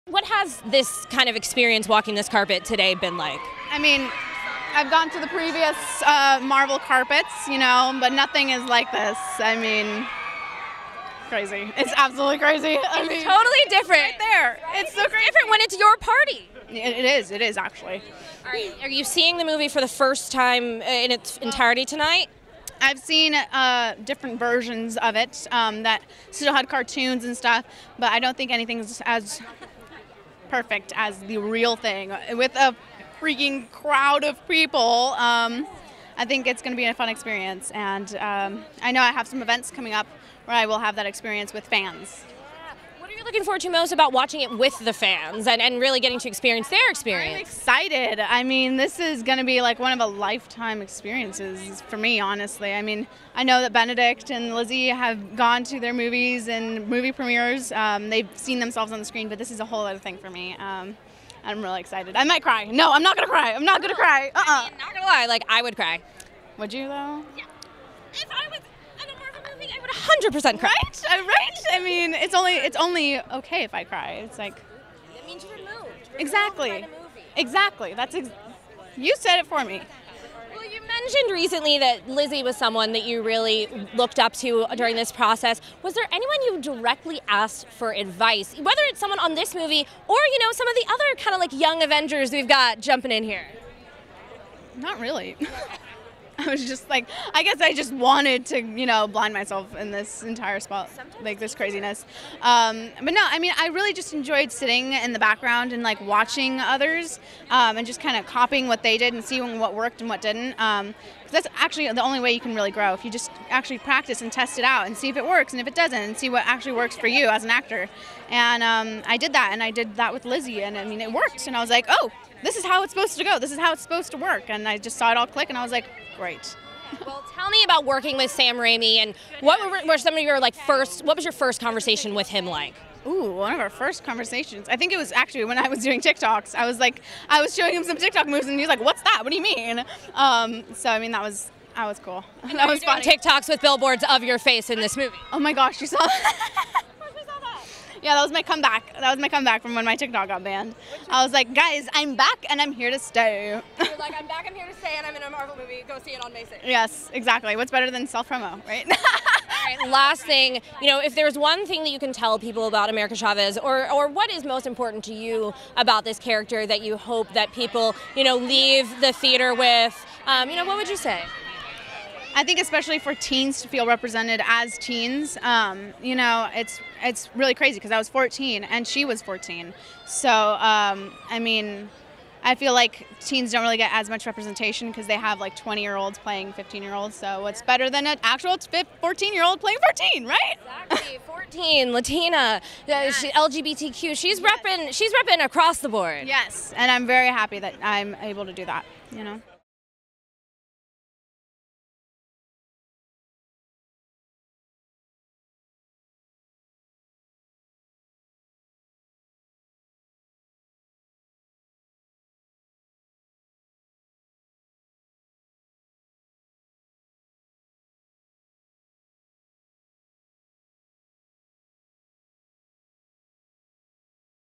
Xochitl Gomez Explains Why She ‘Might Cry’ Watching her MCU Debut as America Chavez
The latest addition to the Marvel Cinematic Universe, America Chavez, is played by Xochitl Gomez who is exceedingly excited.